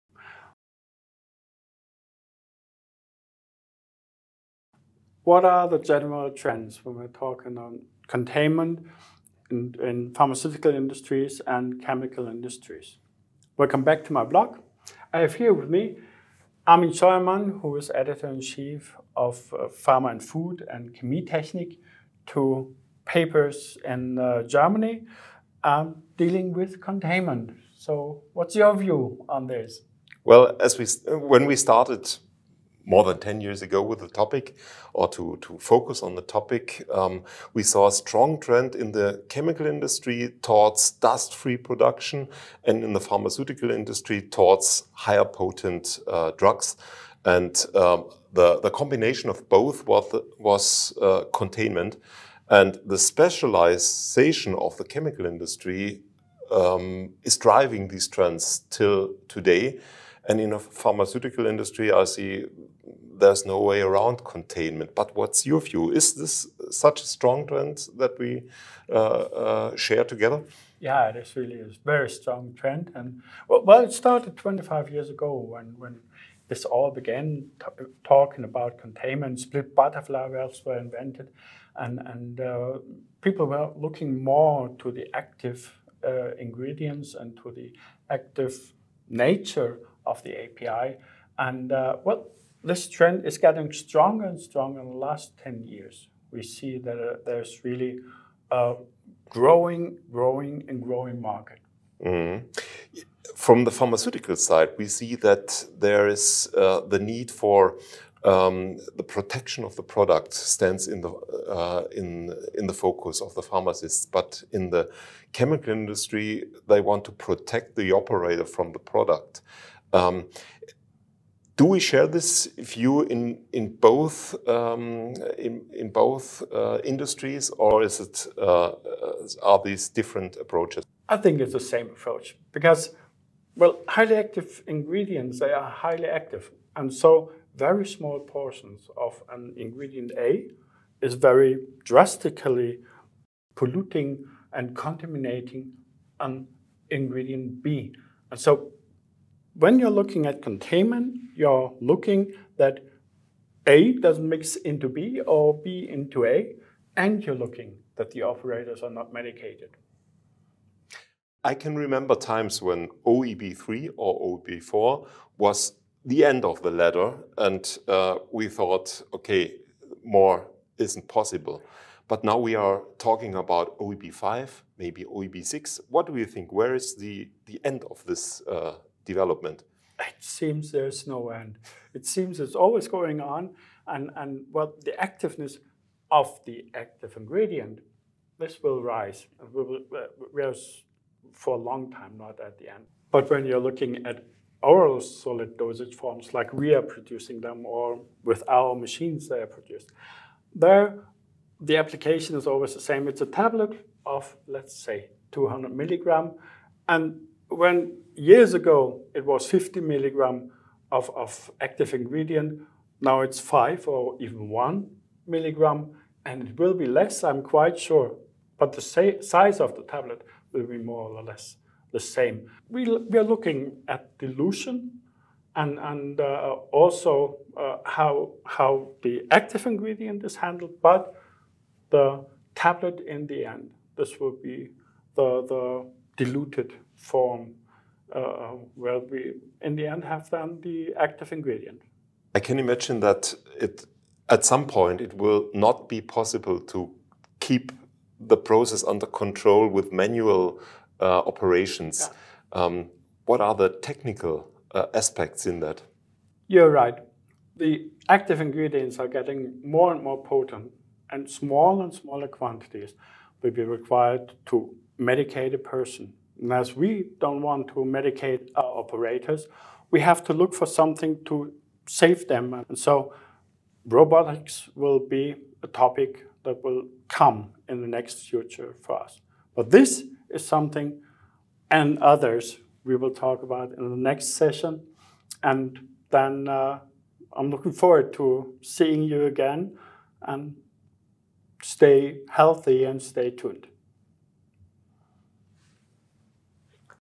Focussing Containment - Episode 07 - General trends in the Pharmaceutical Industry
An interview with Armin Scheuermann, Editor in Chief of CHEMIE TECHNIK and Pharma + Food.

There is a clear trend to higher containment requirements. Both, the Chemical Industry and the Pharmaceutical Industry have to deal with the growing activity of their active ingredients. What is the status of the actual discussion in those industries?